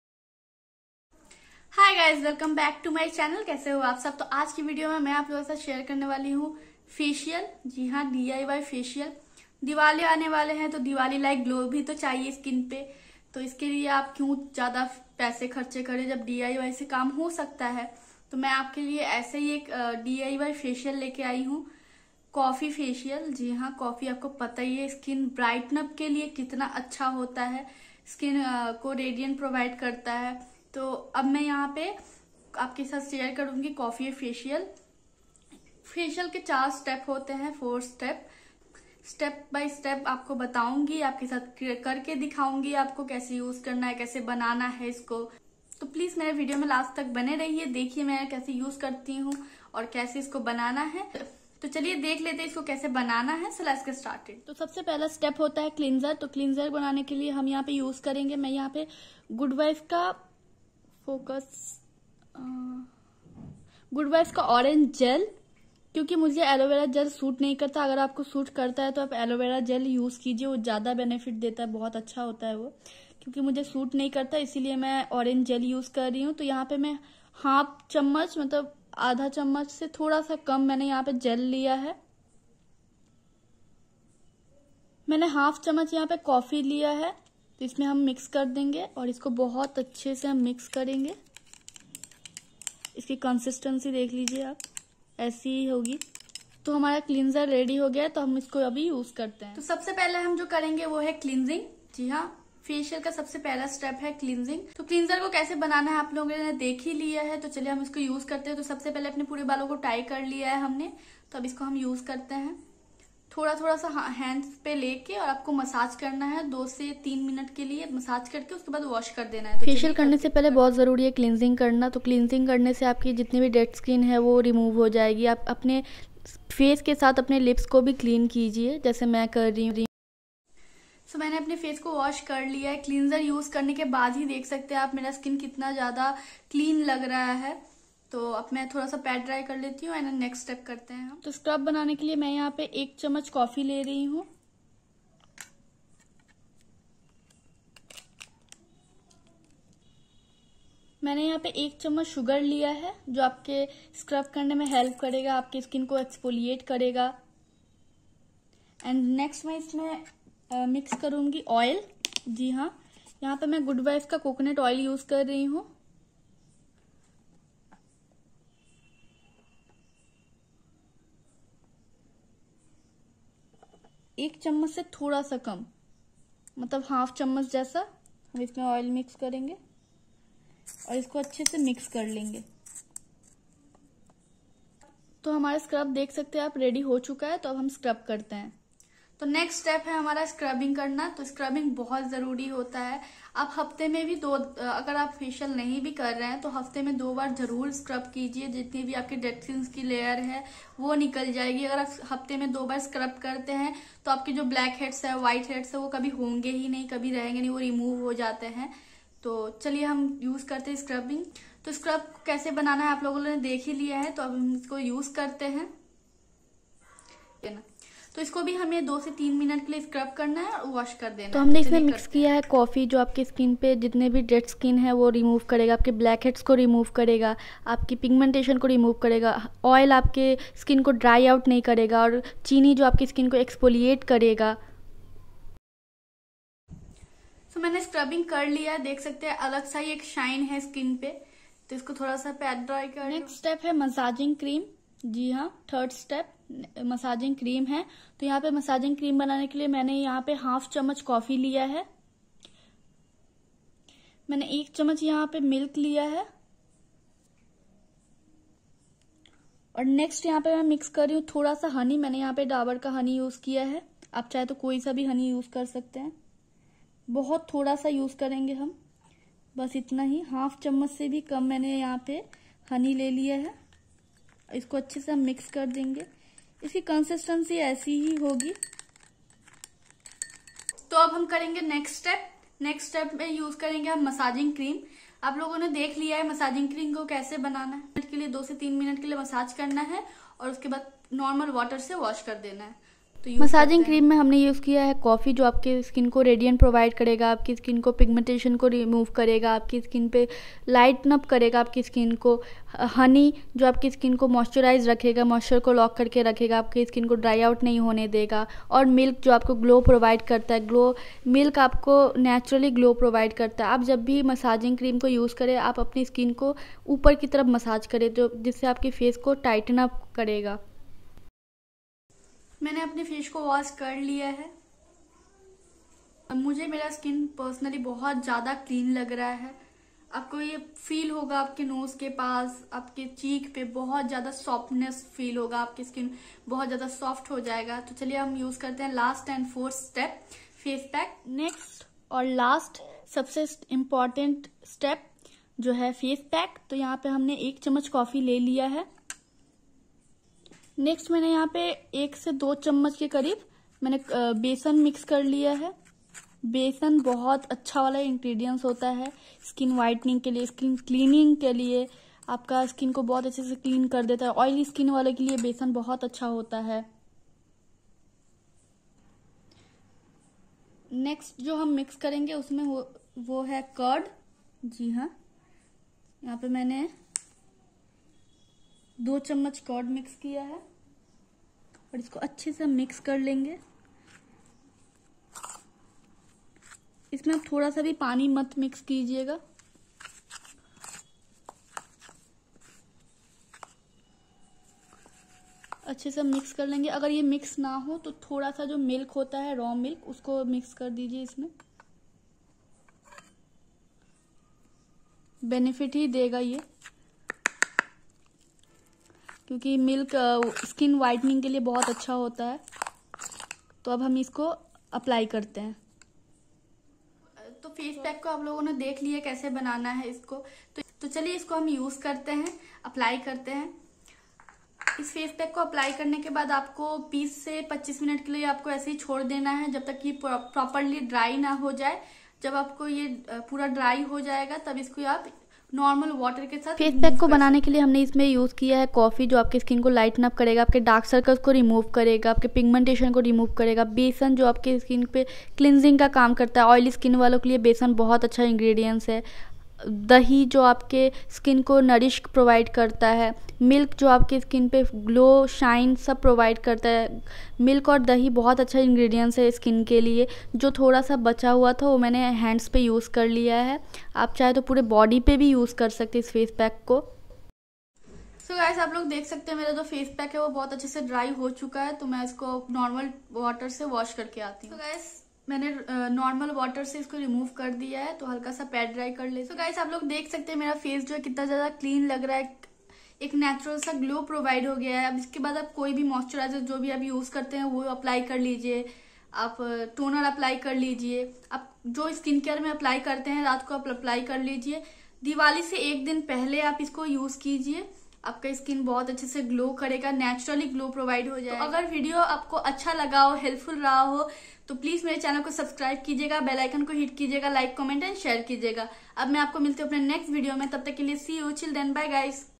parlour like glow at home / DIY coffee facial / diwali special facial /
1) step 1 cleansing
      1tbsp coffee  1tbsp orange gel

2) step 2 scrubbbing
    1 tbsp coffee  1 tbsp sugar  1 tbsp oil

3) step 3 messaging cream
     1 coffee  1 tbsp milk

4) step 4 face pack
     1 tbsp coffee  1 tbsp gram flowr  1 tbsp curd
      1 tbsp milk